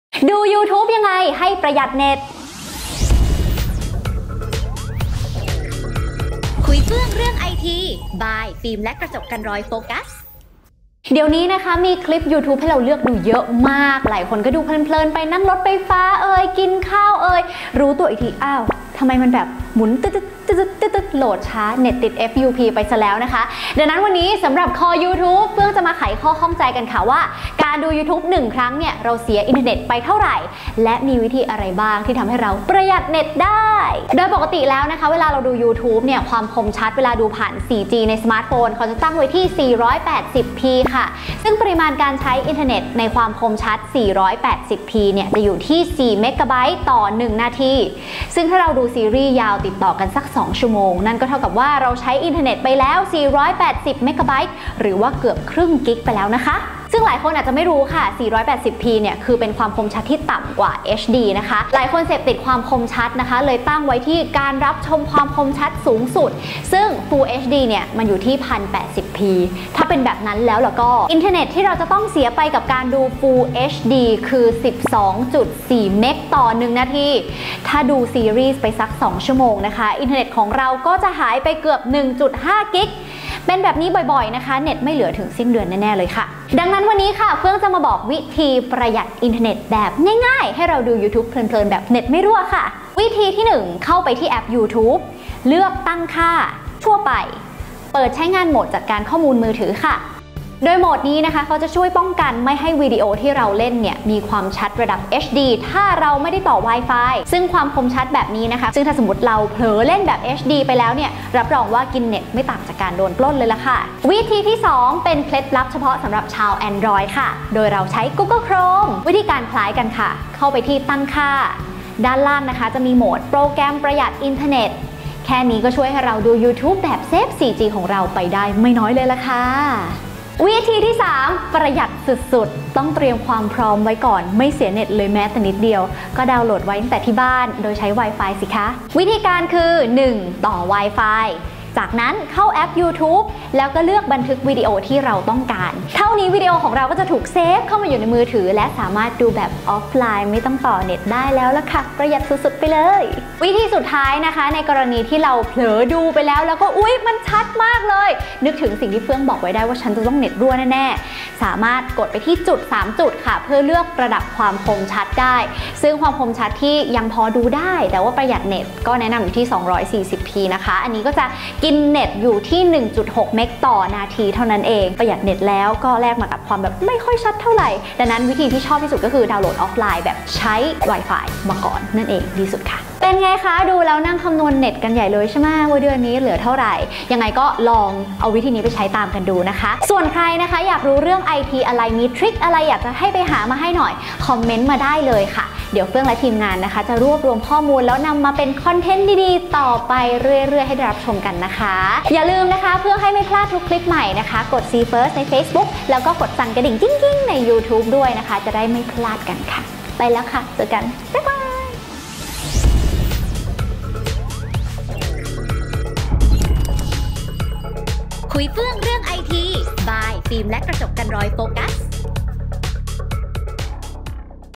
ดู YouTube ยังไงไม่ให้เปลืองเน็ต ! | เฟื่องลดา
ใครเน็ตแห้งเพราะดู Youtube ในมือถือบ้างค้าาา😊

เชื่อว่าหลายคนดู Youtube ในโทรศัพท์บ่อย ดูทั้งวันไม่ว่าจะนั่งรถหรือกินข้าวดูตลอดแต่ใครจะไปรู้ว่าระหว่างที่เราเพลิดเพลินอยู่นั้นได้สูญเสียอินเตอร์เน็ตไปแล้วเท่าไหร่😱

คลิปนี้เฟื่องจะมาบอกว่าทุกครั้งที่เราดู Youtube เราใช้อินเตอร์เน็ตไปเท่าไหร่ และถ้าอยากจะประหยัดขึ้นต้องทำยังไงง 😍